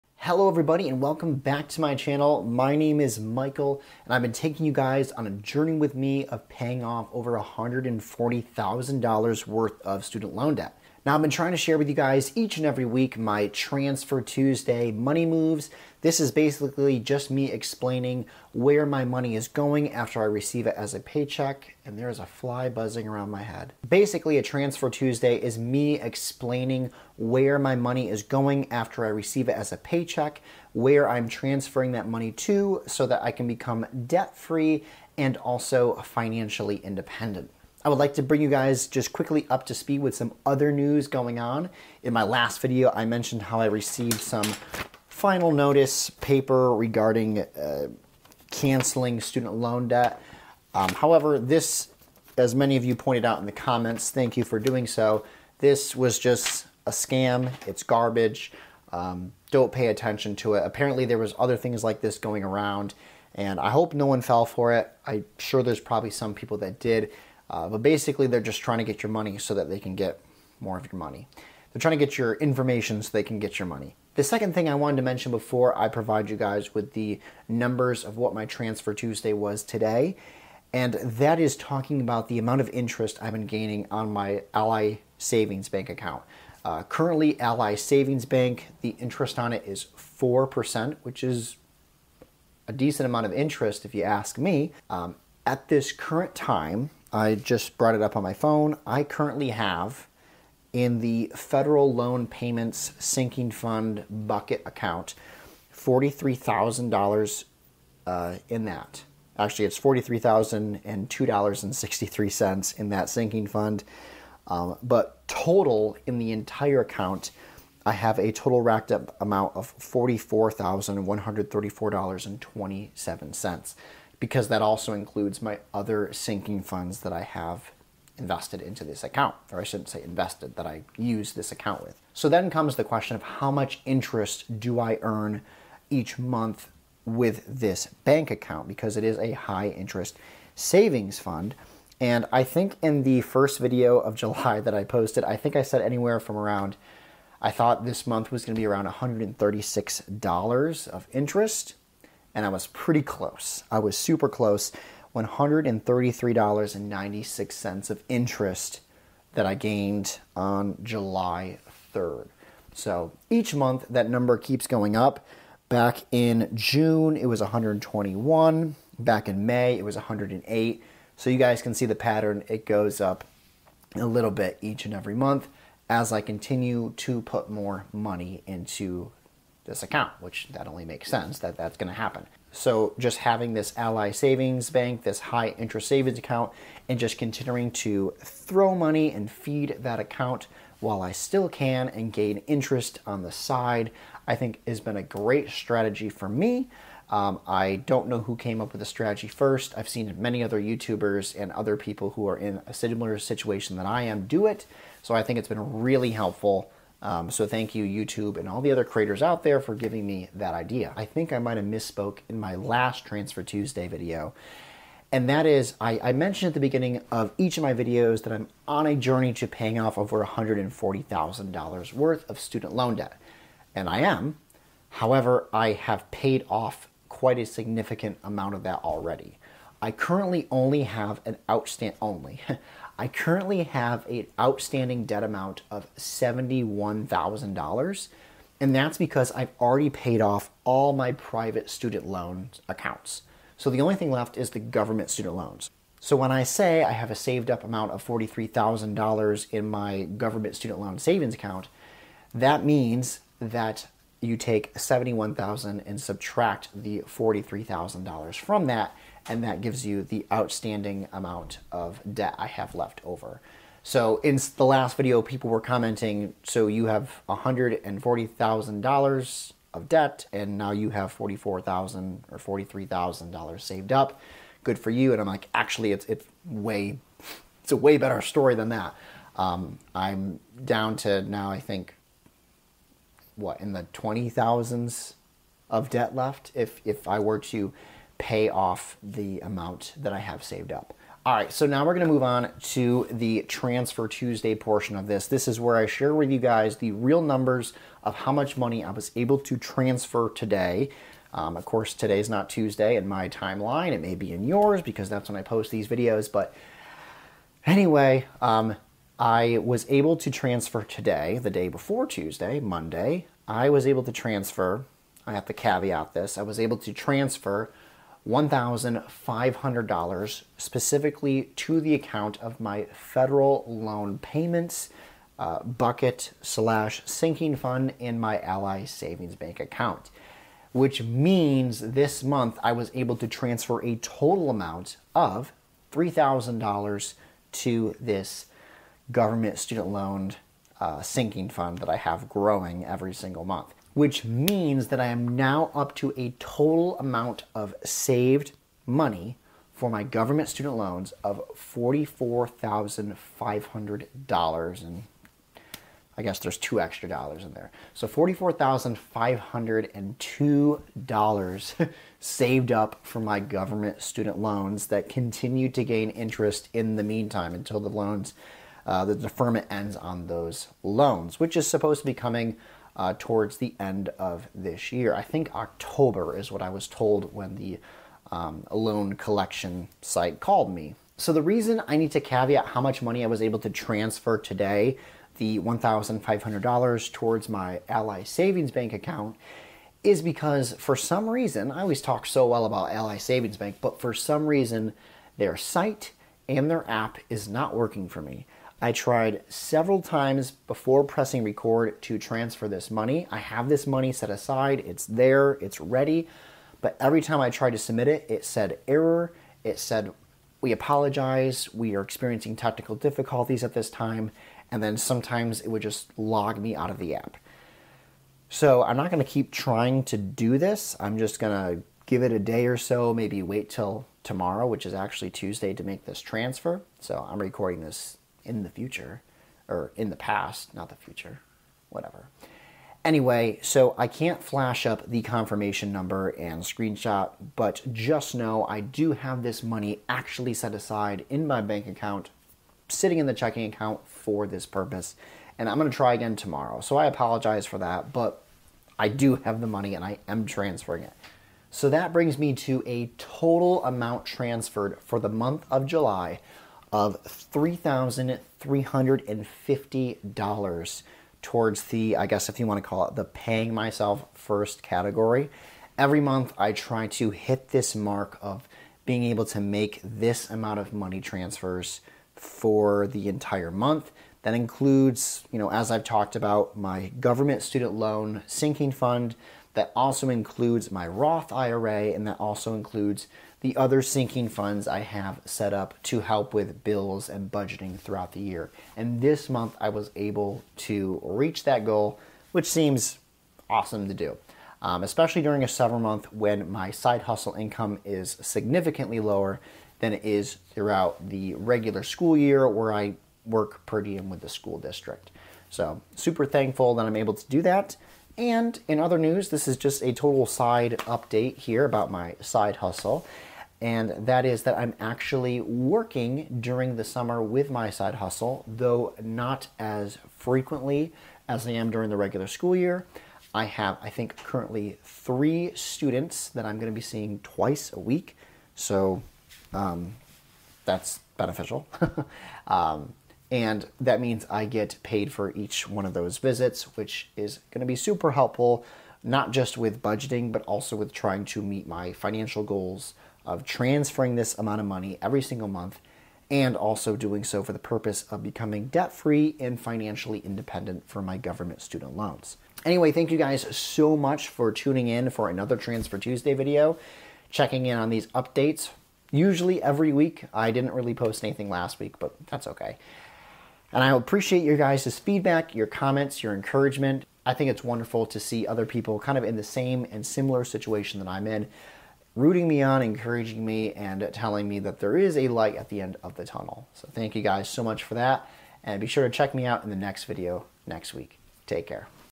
Monthly Goals MET! | July Transfer Tuesday Complete
It sure feels good when you meet your monthly goals of completing a full transfer Tuesday with all the money you said you'd pay yourself with actually going to where it's supposed to!

Making these videos definitely has helped me remain on track with paying off my student loans, tracking my net worth, investing in my retirement, and becoming financially independent and free! :)